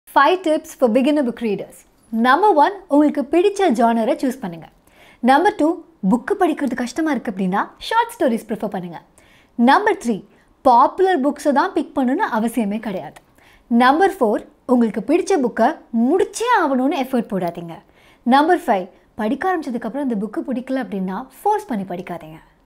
5 Tips For a Beginner Book Readers 📚 | Steps to read books | The Book Show ft. RJ Ananthi
You can find me on Radr from now on!
The Book Show, presents you with a wide range of informative yet witty content.

Follow Us On:

The Book Show